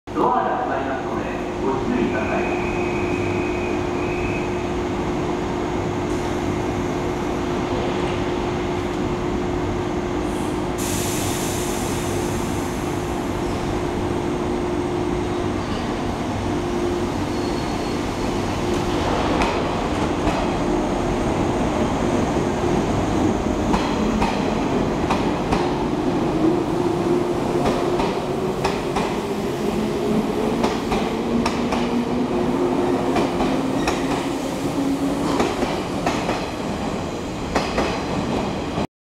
485系特急北越3号新潟行き信越本線長岡駅発車[国鉄色]
485系特急北越3号新潟行き長岡駅発車[国鉄色]
・485系新ニイK1編成